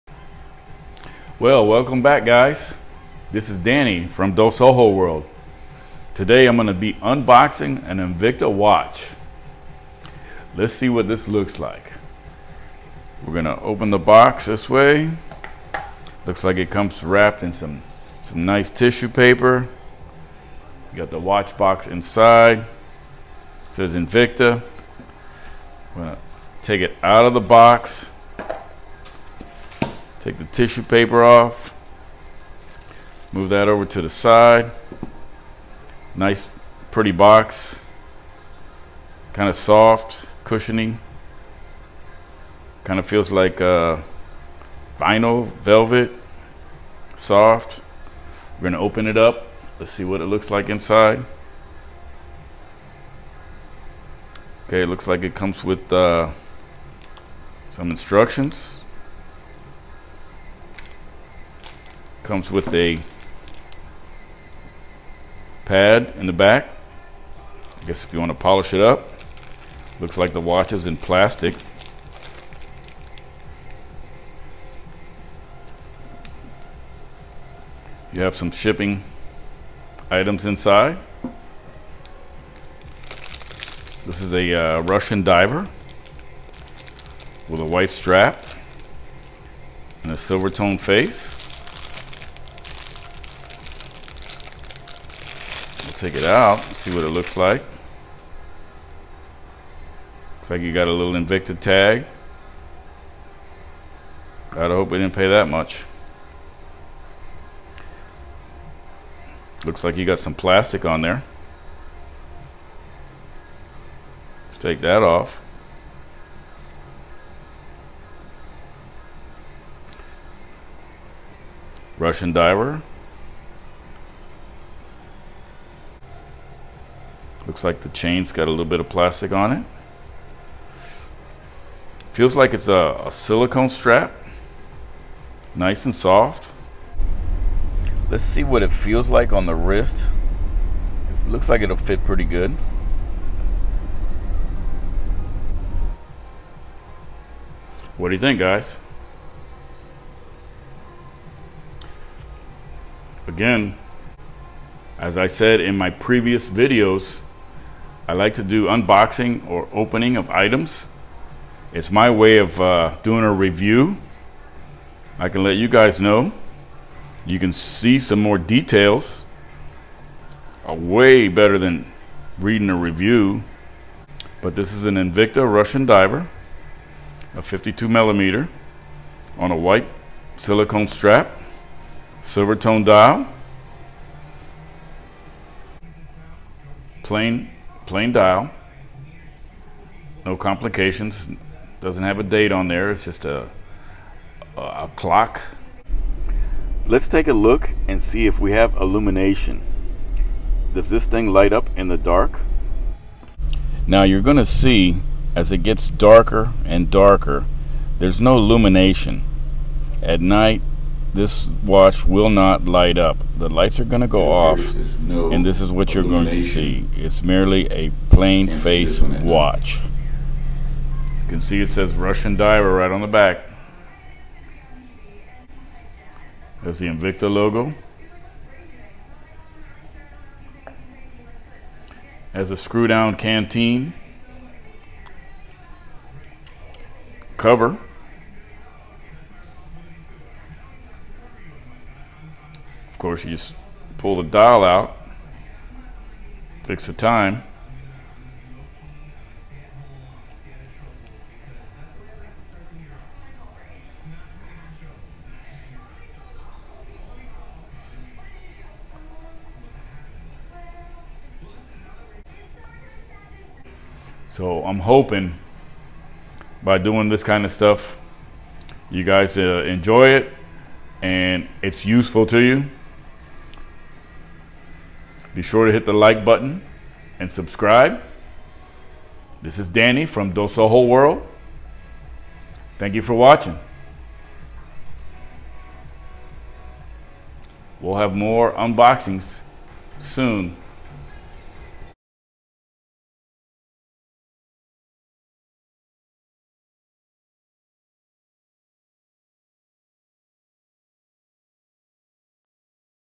INVICTA RUSSIAN DIVER
I’m boxing an Invicta Russian diver watch any music you may hear on this video is not mine and I do not endorse any products you may see in any video